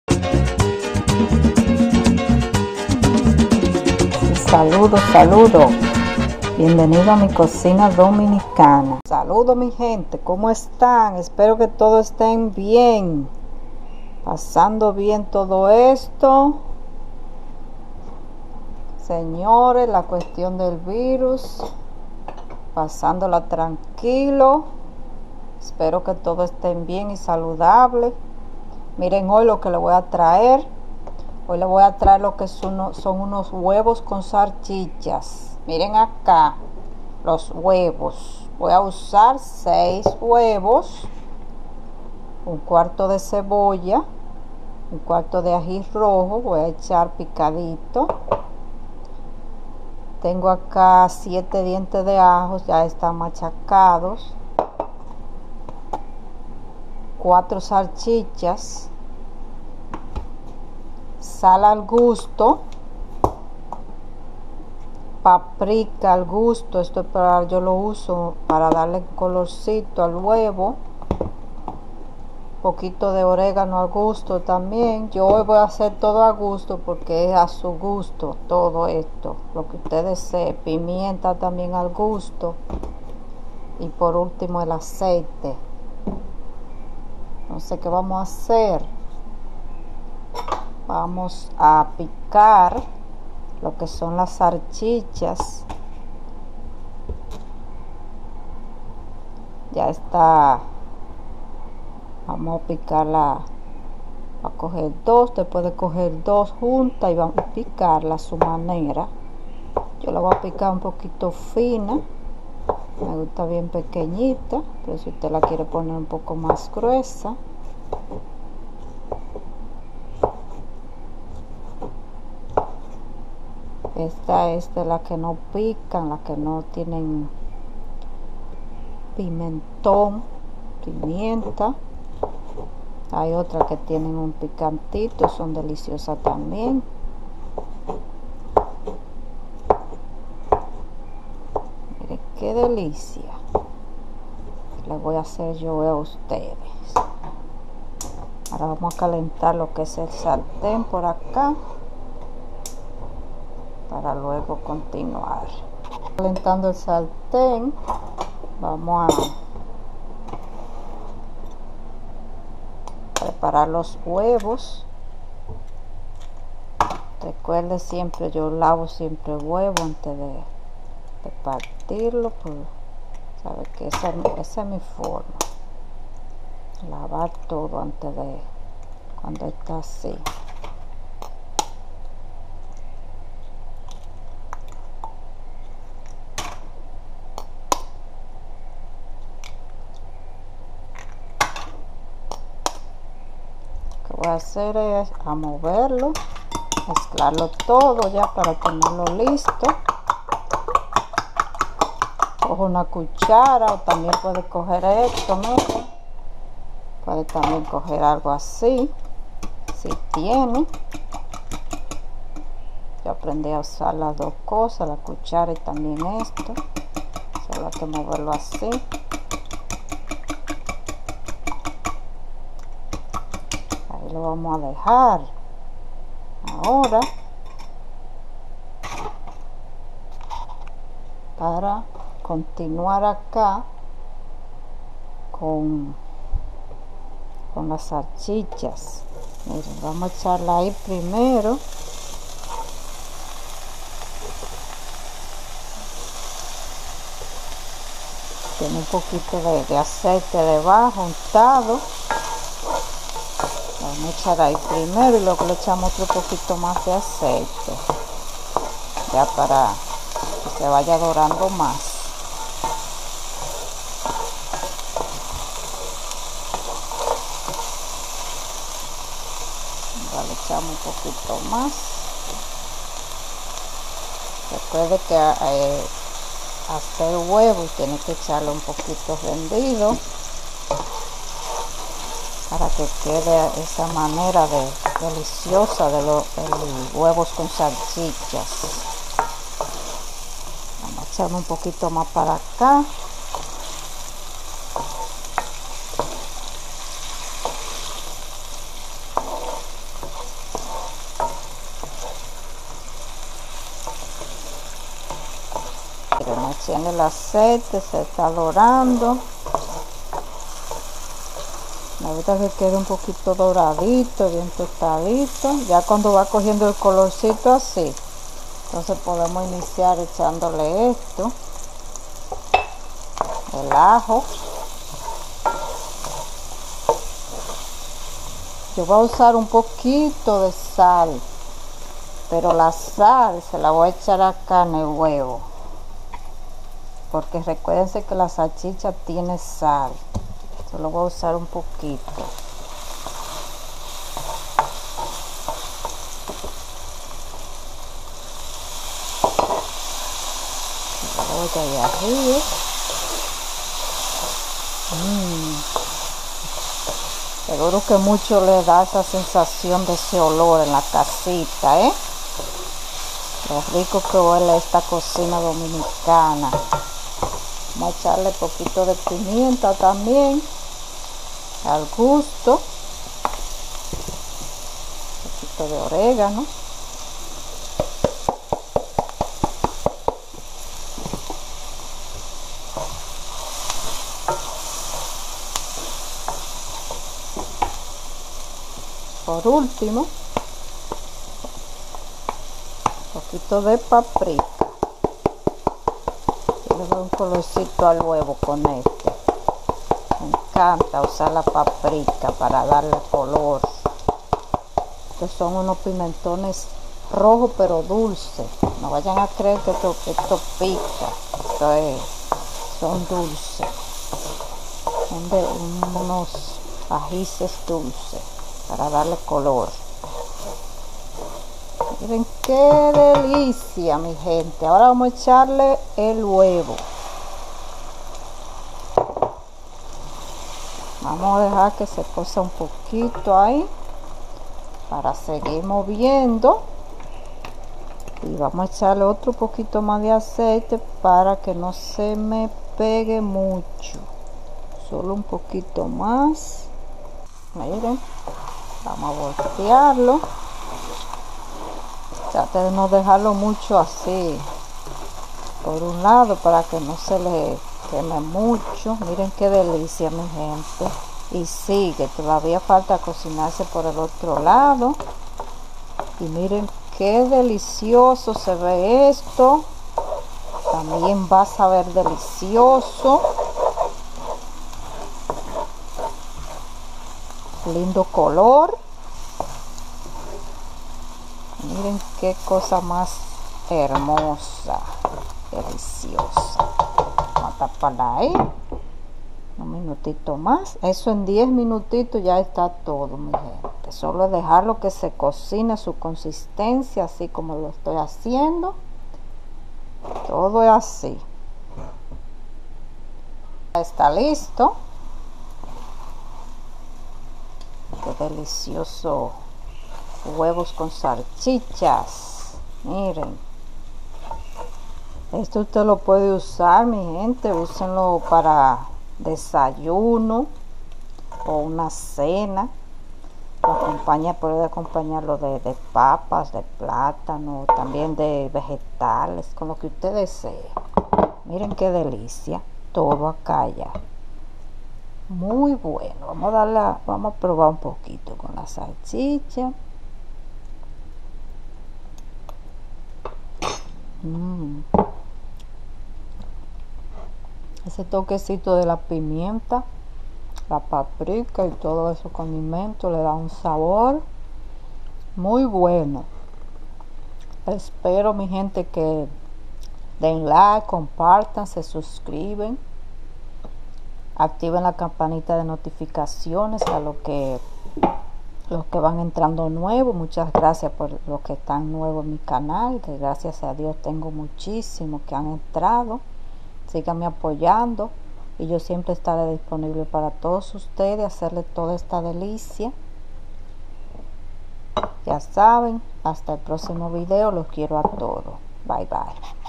Huevos revueltos con salchichas
Ingredientes:6 huevos,1/4 de cebolla,1/4 de aji rojo,7 dientes de ajo,4 salchichas,sal al gusto,paprika al gusto,oregano al gusto,aceite y pimienta al gusto